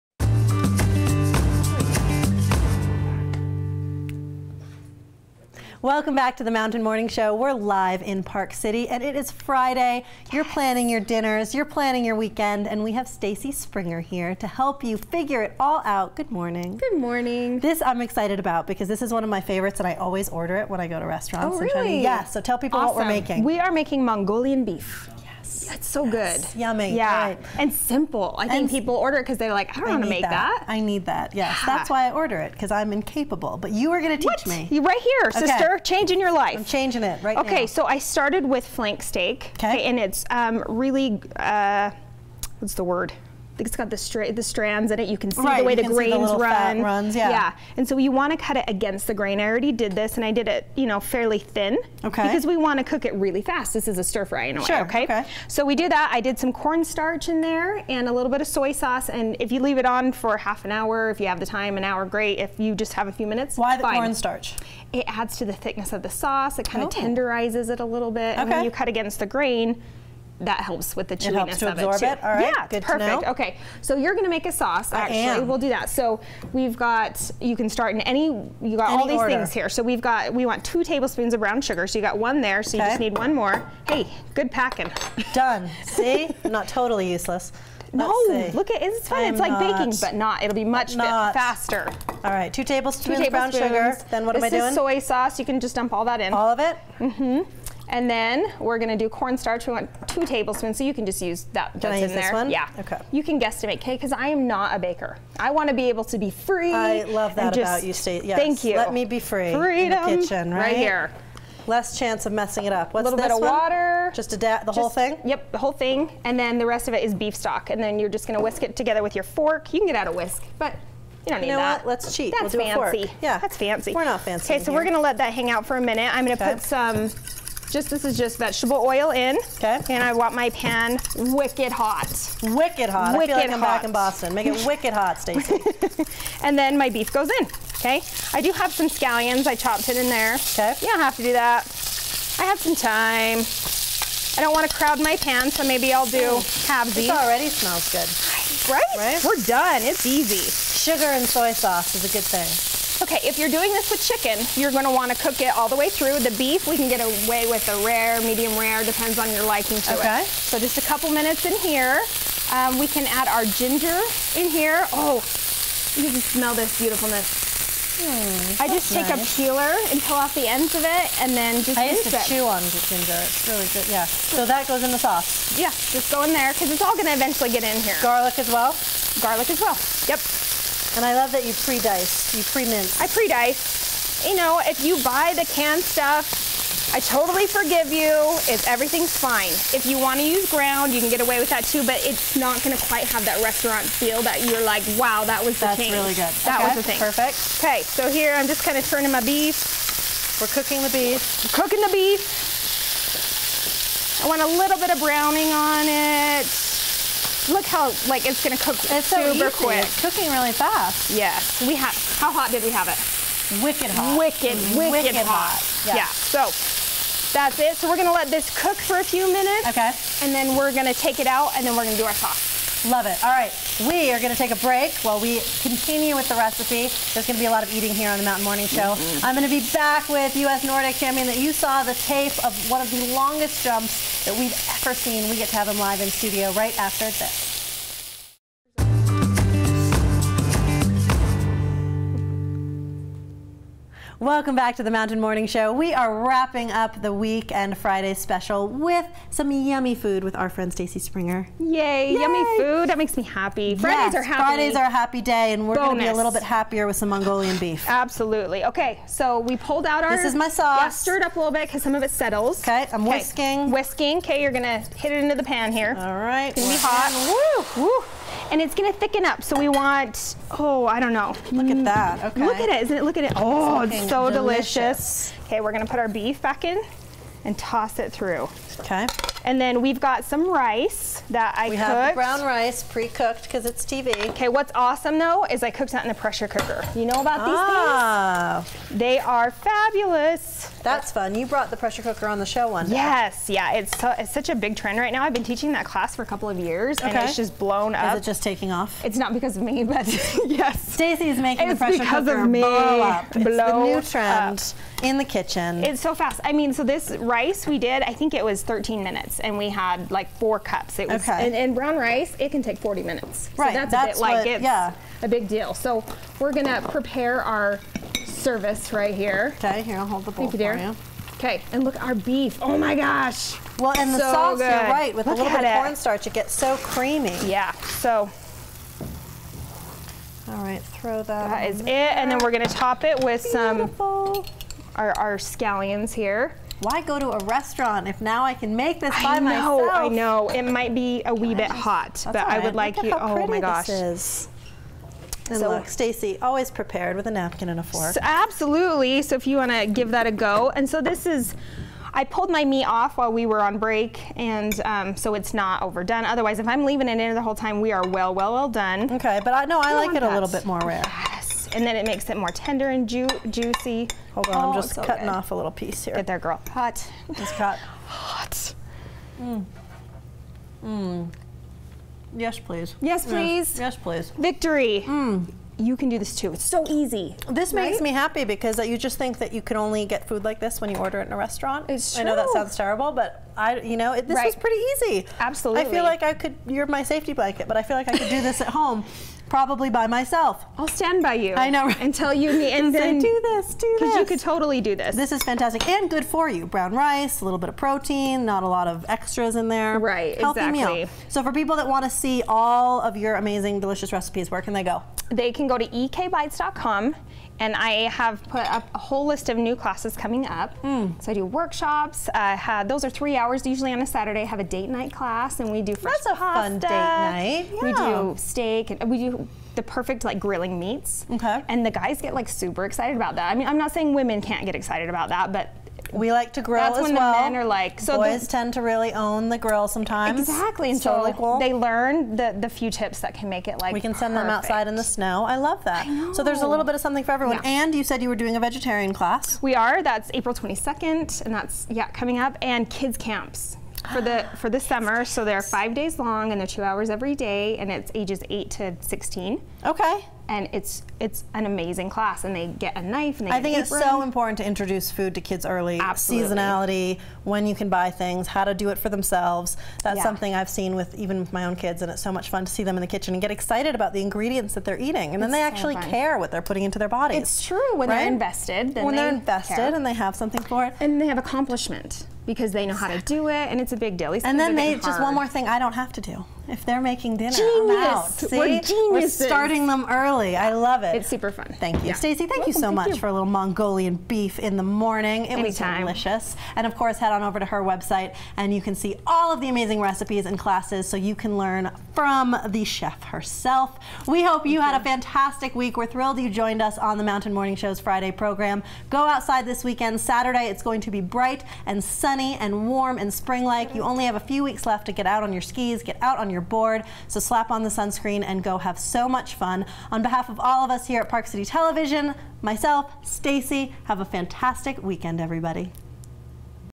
Einstein's Kitchen with Stacy Springer, Mongolian Beef
Stacy Springer cooked up some tasty treats for us today and shared some tips to make your at home meals shine like pro-restaurant quality. check out more tips from Stacy at ekbites.com